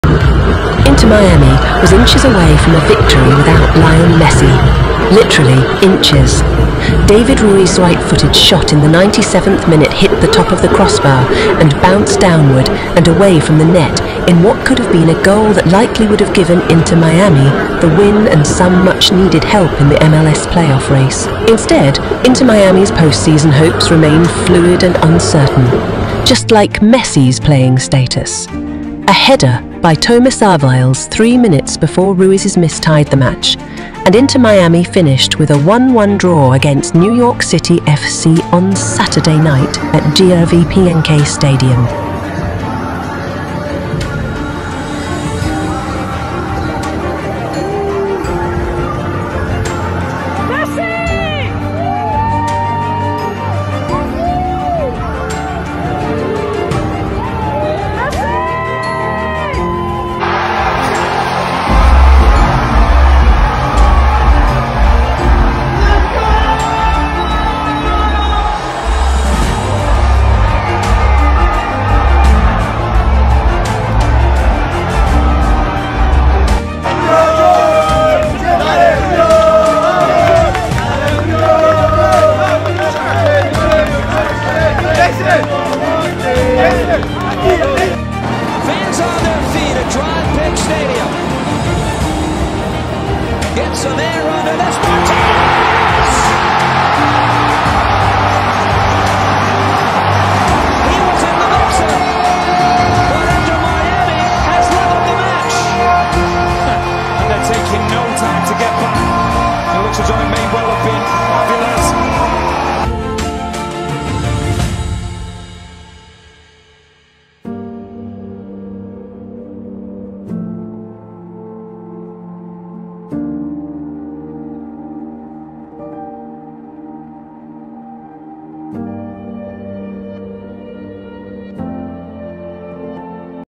Messi’s INSANE Reactions to Inter Miami’s Performance vs New York City FC
Messi’s EPIC Reactions to Inter Miami’s Performance vs New York City FC